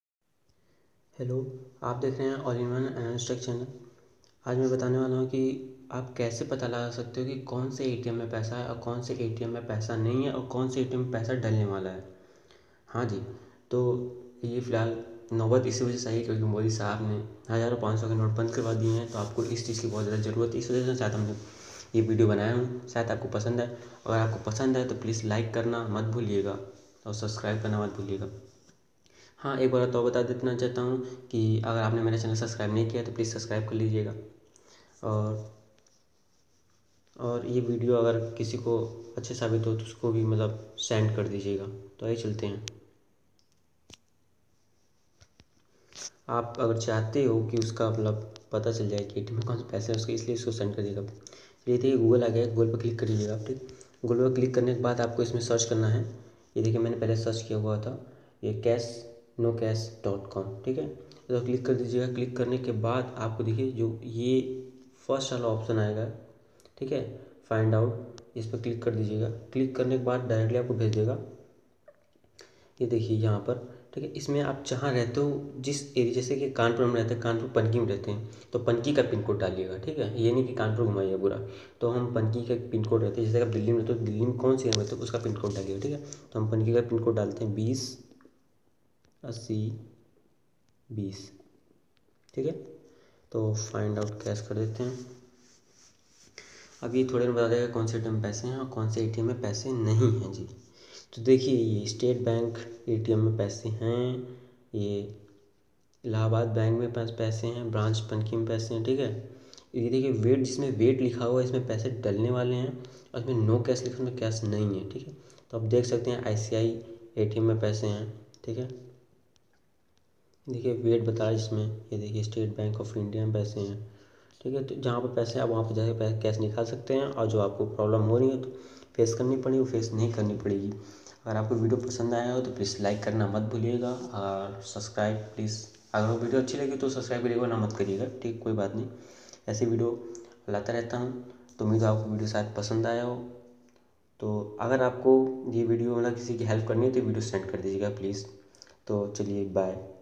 How to Find an ATM with Cash Using an Online web site
Hello friends aap dekh rhe h ,all in one Anuj tech , channel and mai aaj aap ko es  video me ye batane Wala hu Ki aap Kaiser pata laga skte h Ki kon se ATM me paise h ya nahi h ya phir paise dalejane wale h .
To watching this video How to Find an ATM with Cash Using an Online web site